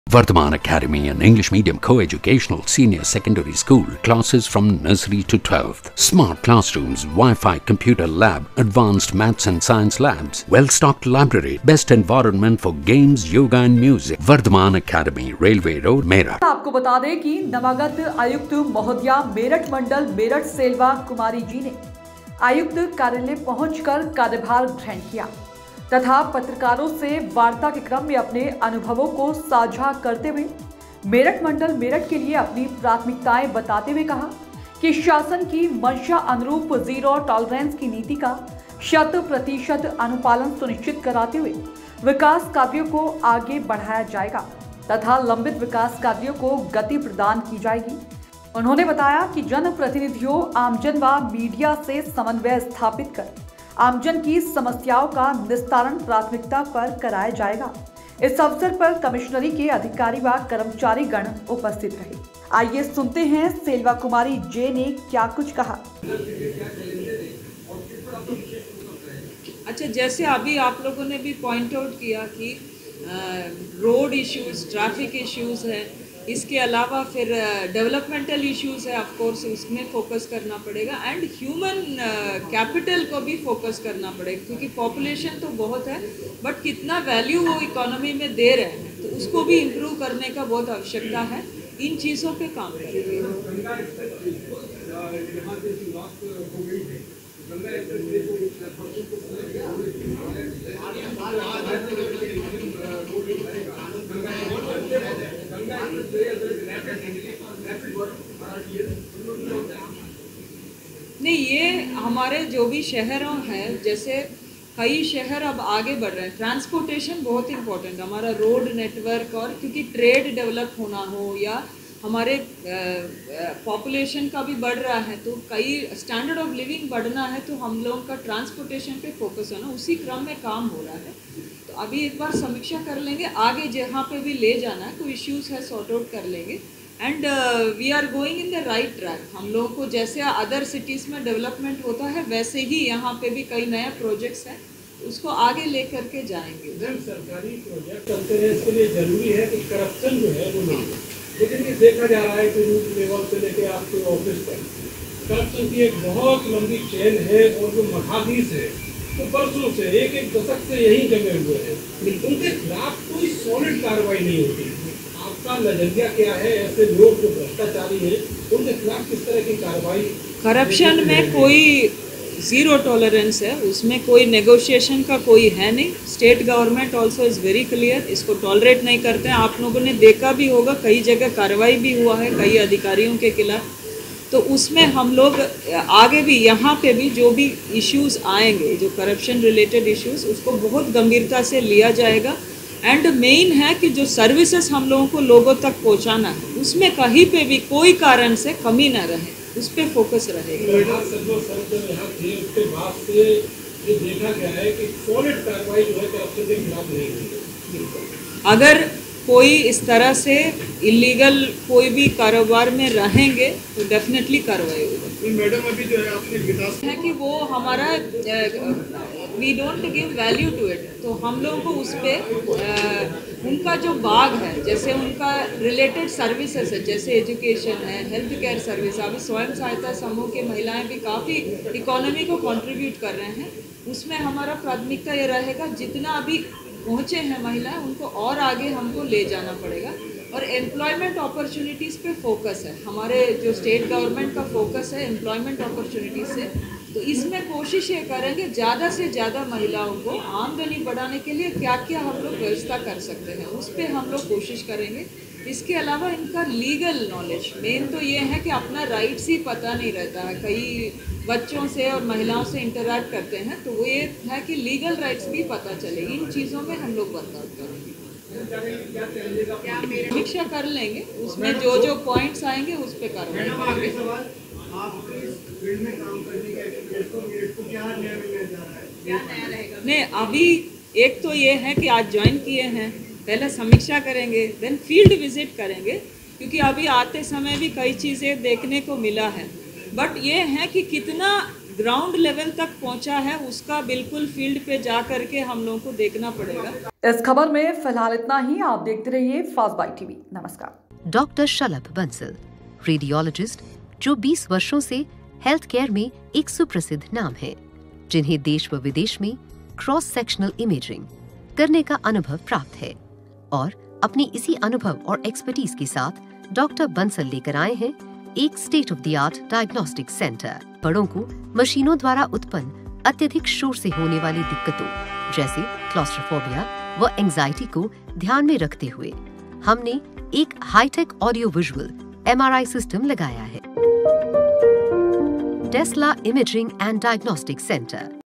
सेल्वा कुमारी जे ने नवागत आयुक्त मेरठ मंडल मेरठ का कार्यभार ग्रहण किया || First byte.tv
नवागत आयुक्त मेरठ मंडल मेरठ ने किया कार्यभार ग्रहण

नवागत आयुक्त,मेरठ का पद ग्रहण किया सेल्वा कुमारी जे ने

शासन का अनुपालन सुनिश्चित कराते हुये विकास कार्यों को आगे बढाया जायेगा

इस अवसर पर कमिश्नरी के अधिकारी व कर्मचारीगण उपस्थित रहे